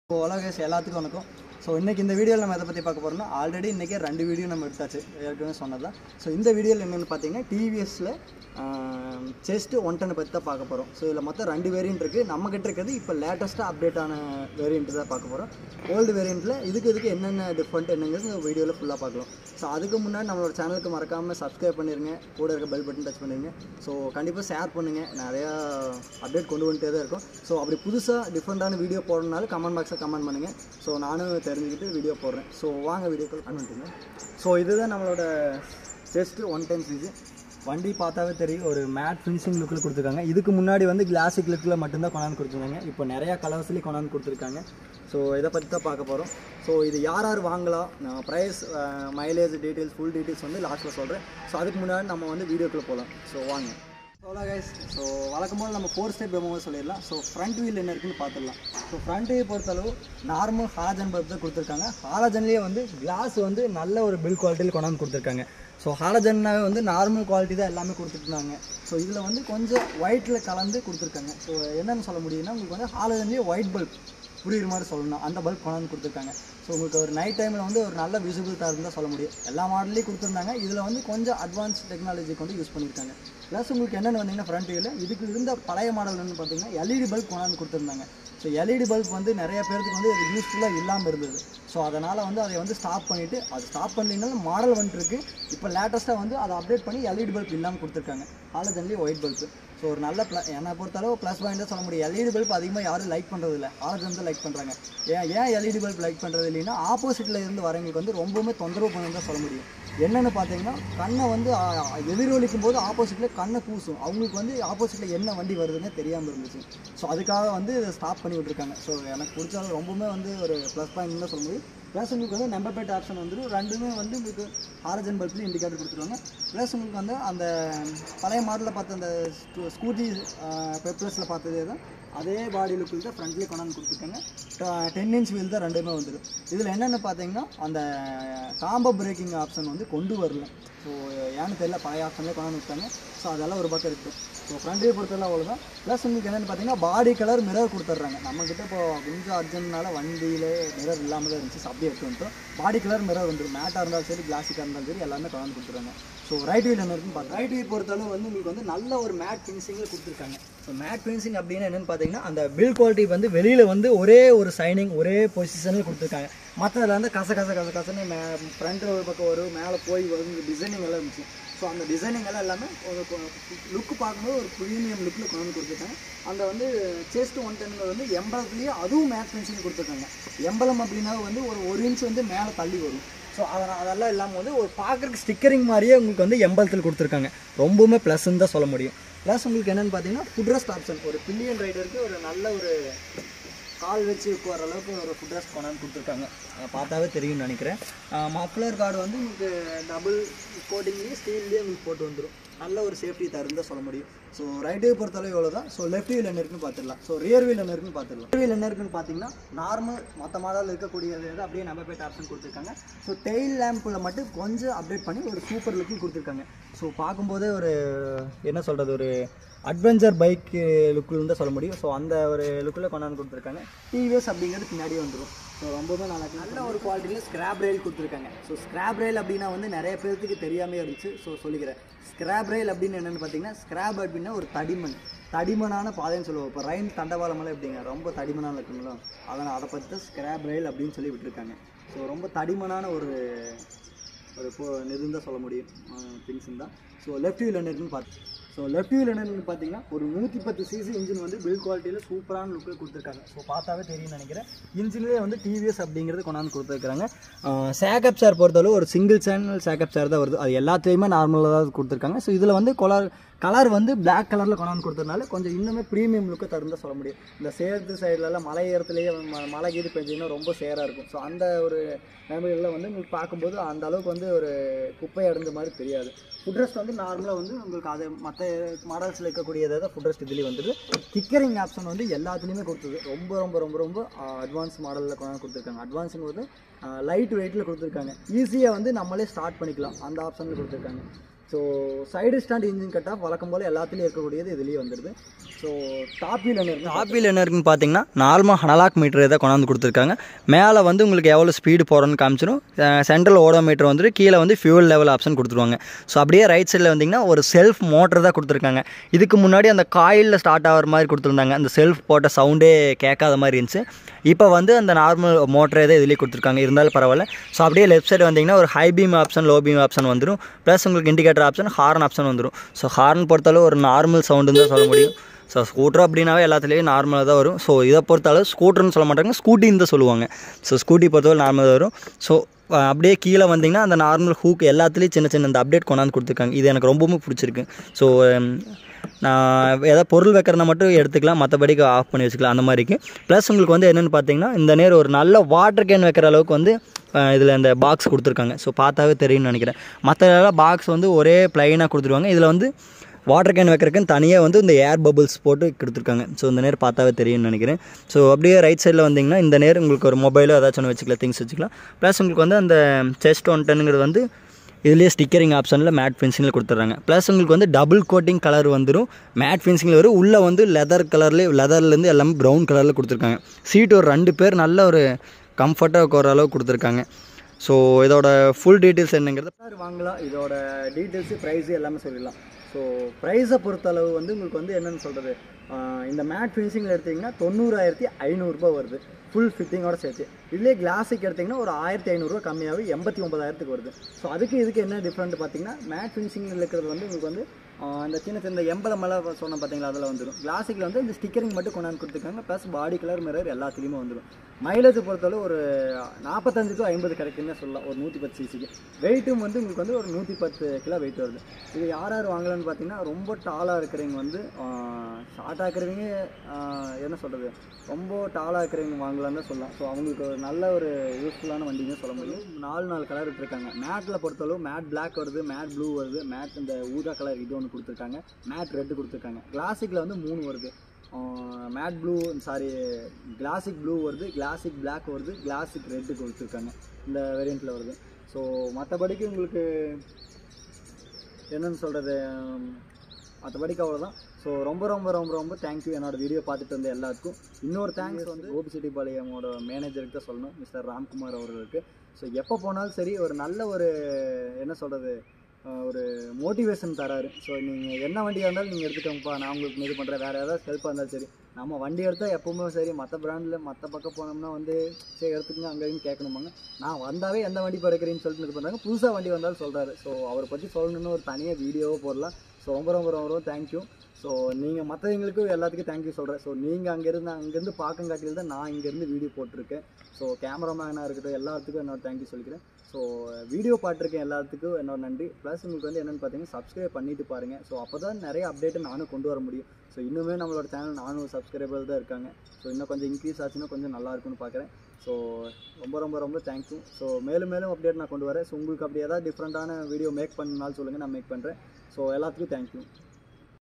TVS ZEST 110 FULL Review in Tamil 🤓| All marketing field best mileage scooter 🤔
S V TVS, GOBICHETTIPALAYAM

SALES MANAGER : MR. Raam Kumar